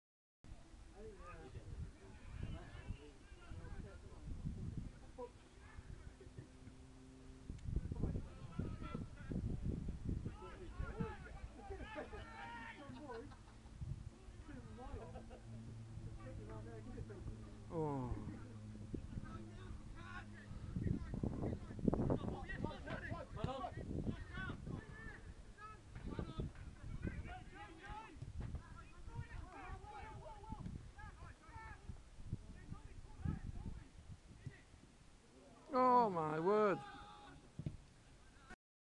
ECPL Premier League Game ,S Roche 0 v 1 t Stephen played yesterday 09/09/20
St Stephen go close again